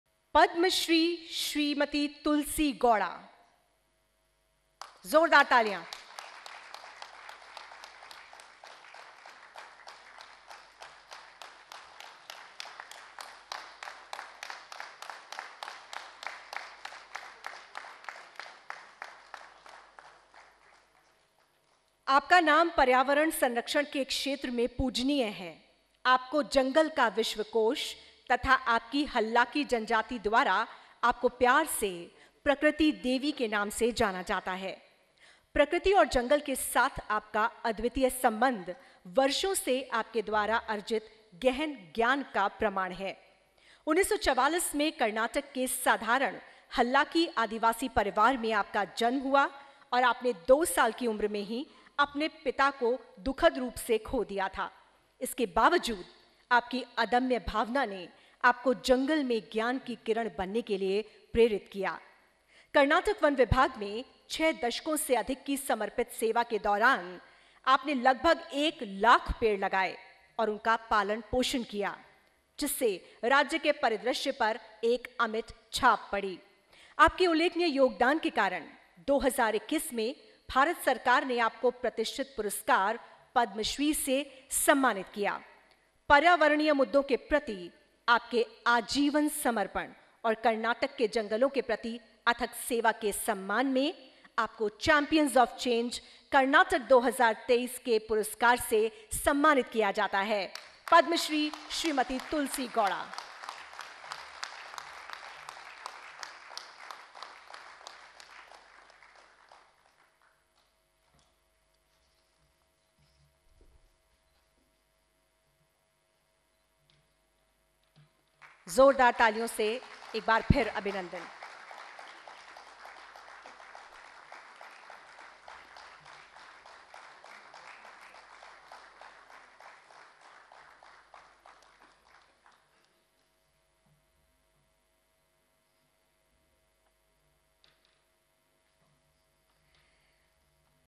"Padma Shri Smt. Tulsi Gowda: Karnataka's Champion of Change"
🌿 Join us in honoring the incredible Smt. Tulsi Gowda, a Padma Shri awardee and a leading Indian environmentalist, for her well-deserved recognition as a CHAMPION OF CHANGE in Karnataka! 🌟 Her unwavering commitment to environmental conservation is a shining example for all. She not only uplifts Karnataka but also inspires positive change in Bangalore and the world. 🌍💚🏆 Let's celebrate this remarkable ChampionOfChange!